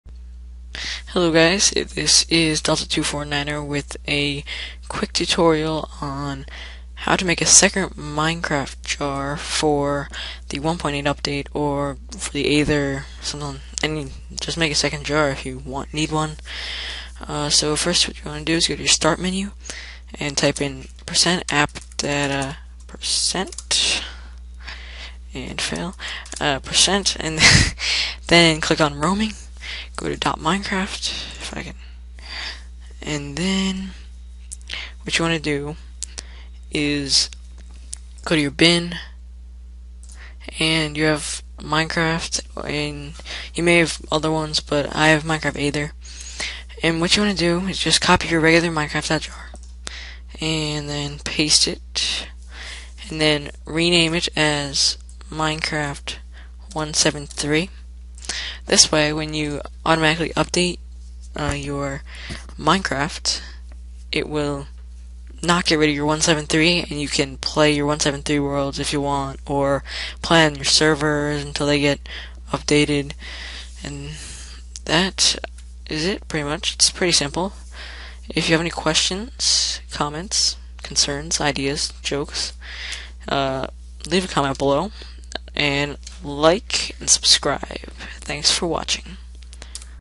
How to Make a Second Minecraft Jar
Quick tutorial on how to make a second minecraft.jar for the 1.8 Update.

Here's the tutorial in text:

1. Click on your start menu
2. Type in "%appdata%" (no quotes)
3. Click Roaming, then .minecraft
4. Navigate to the bin folder
5. Copy your current minecraft.jar and paste it. (should paste as minecraft.jar - Copy)
6. Rename it as minecraft173.jar